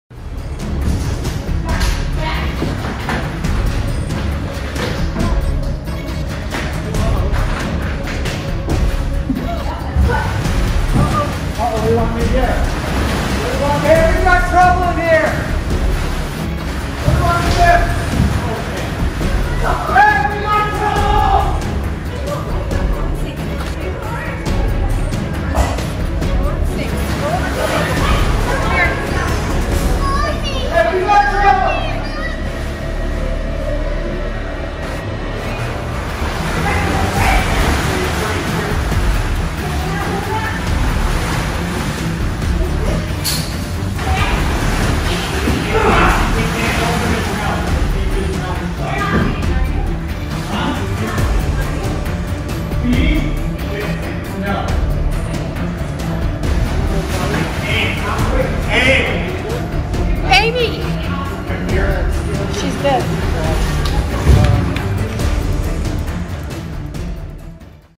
惊险！鳄鱼表演时突然咬住驯兽员，一名围观孩子父亲果断相救！
近日，在犹他州西瓦利市（盐湖城郊区）的爬行动物中心，一名客人跳入围场，以营救一名在儿童生日派对上被鳄鱼拉进来的驯兽员，现场惊险万分。